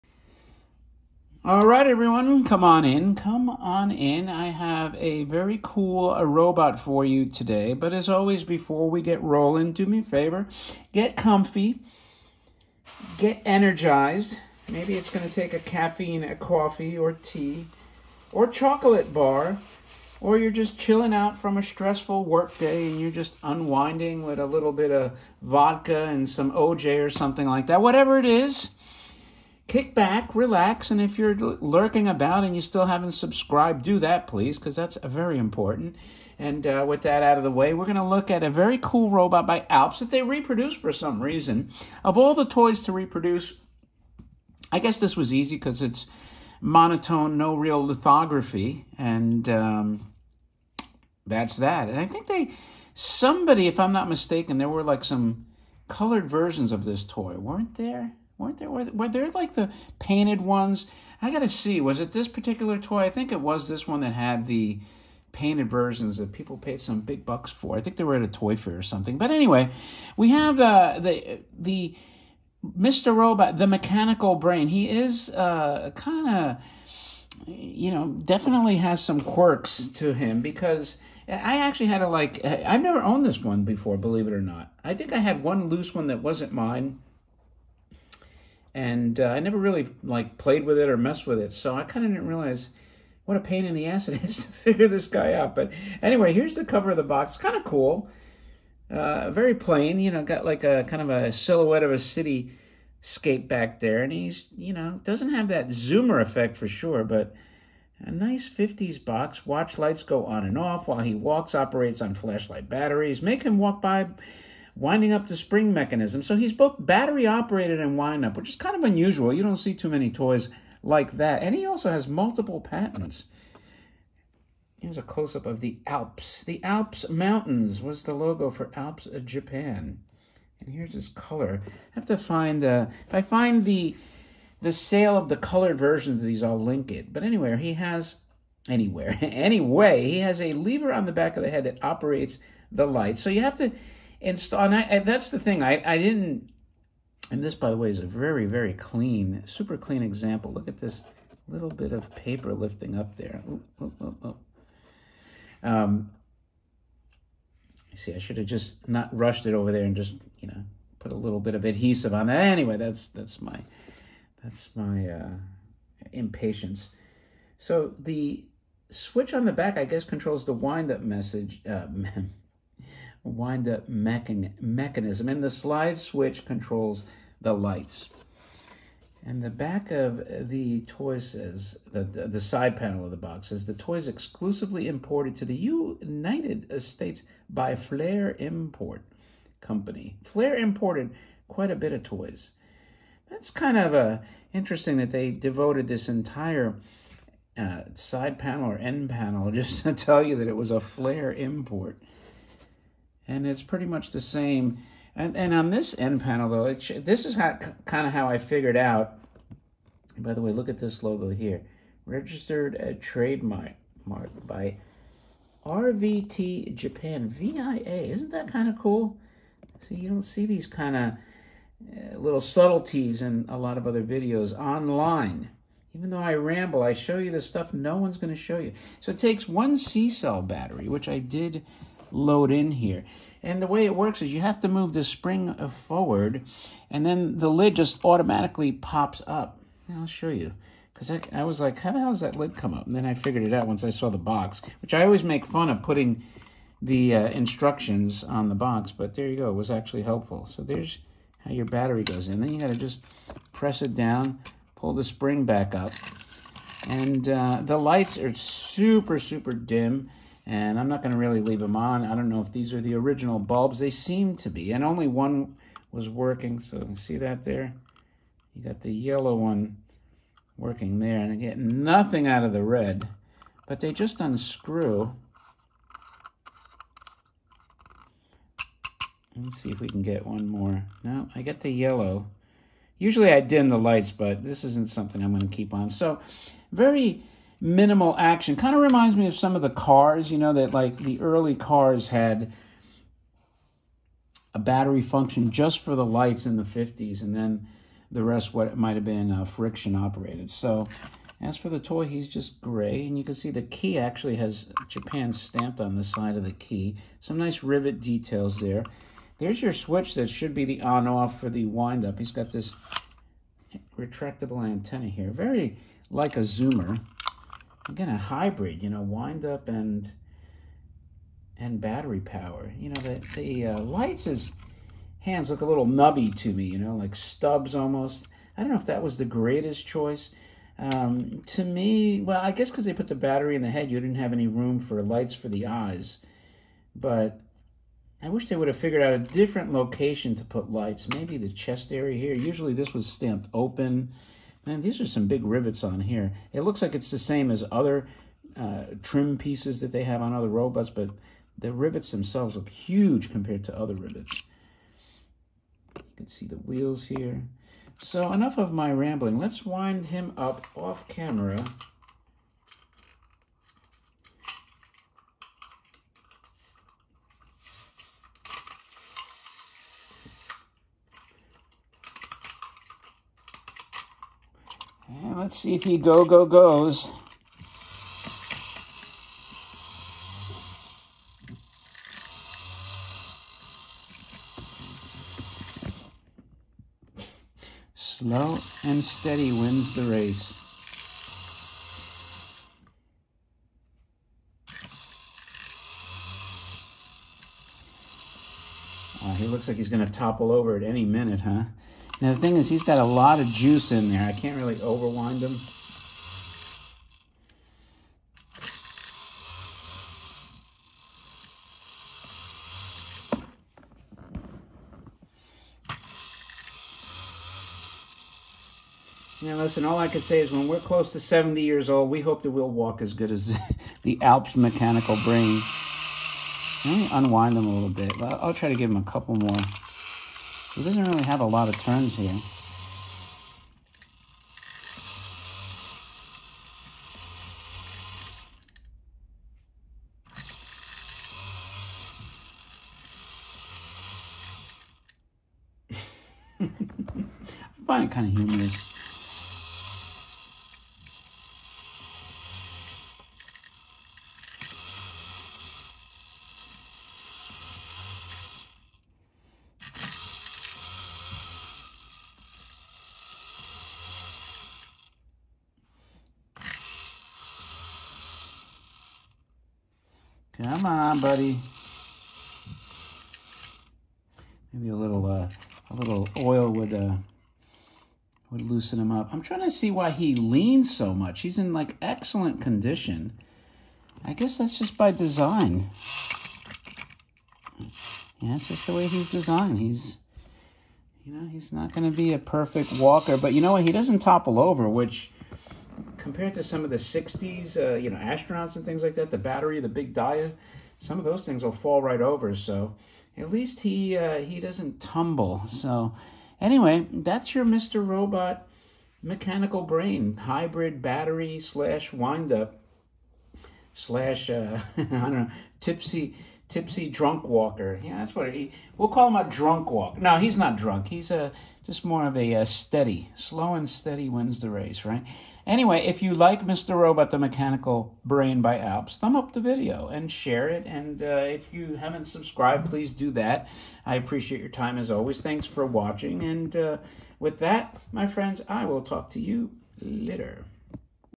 Mr Robot The Mechanical Brain is a battery powered Wind Up! 🙀
1950’s Alps Japan